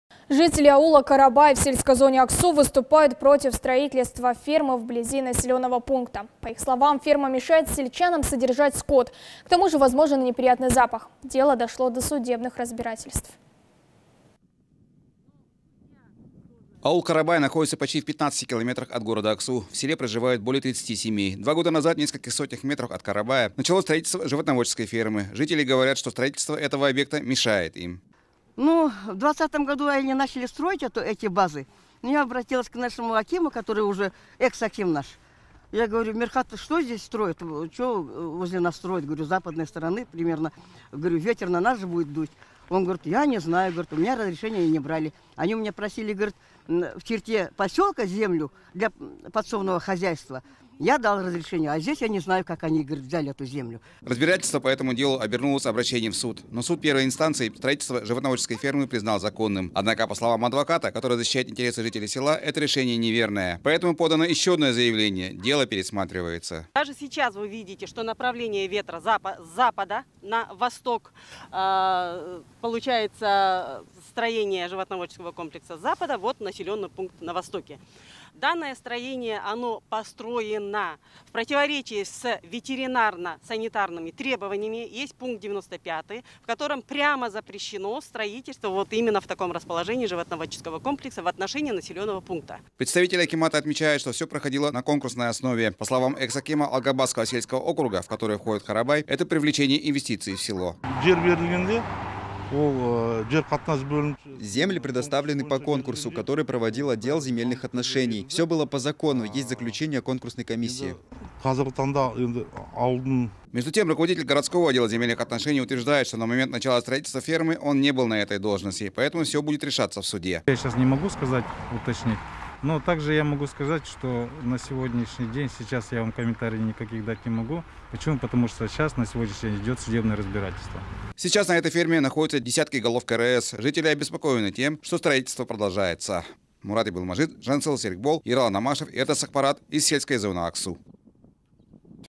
И снова дело в ферме
Жители аула Карабай в сельской зоне Аксу выступают против строительства фермы вблизи населенного пункта. По их словам, ферма мешает сельчанам содержать скот, к тому же возможен и неприятный запах. Дело дошло до судебных разбирательств.